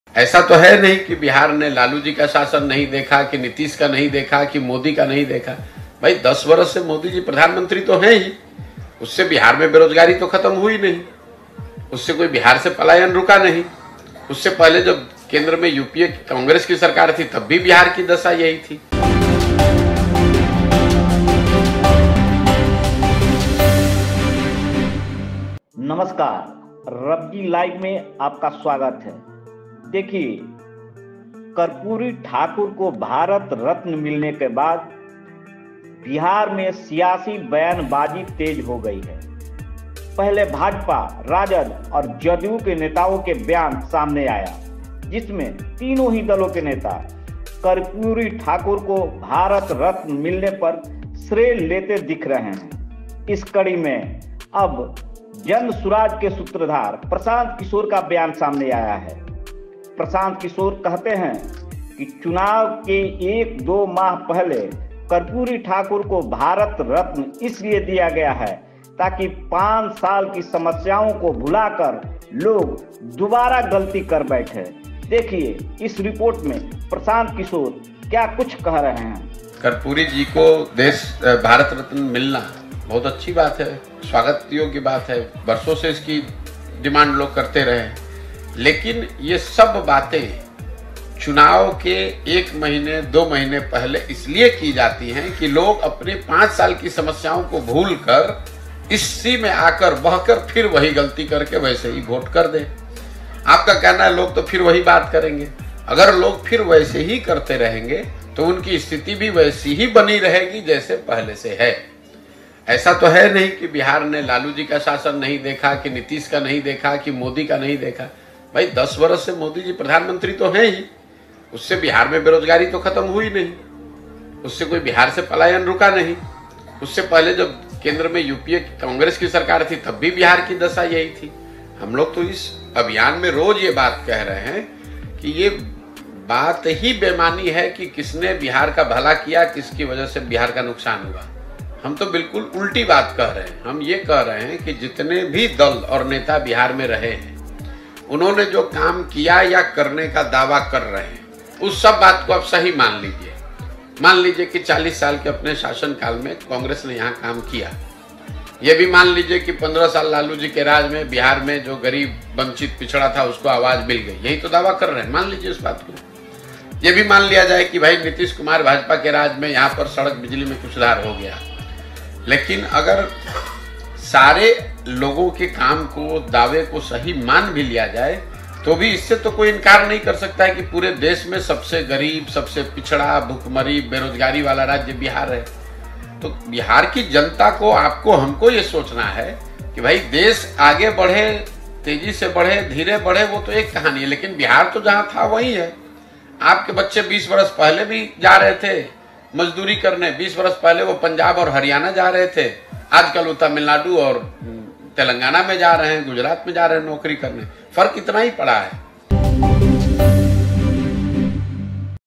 Karpoori Thakur को भारत रत्न मिलना स्वागत योग्य बातें : Prasant Kishore
जन सुराज पदयात्रा के सूत्रधार प्रशांत किशोर ने कहा कि कर्पूरी जी को भारत रत्न मिलना बहुत अच्छी और स्वागत योग्य बातें हैं। इसकी बिहार और देश में बहुत लंबे समय से मांग की जा रही थी। लेकिन, ये सब बातें चुनाव के 1 से 2 महीने पहले इसलिए की जाती हैं ताकि लोग अपनी 5 साल की समस्या को भूलकर दोबारा इन्हीं सब बातों में आकर बहकर फिर से वही गलती कर दे और उन्हें ही वोट कर दे।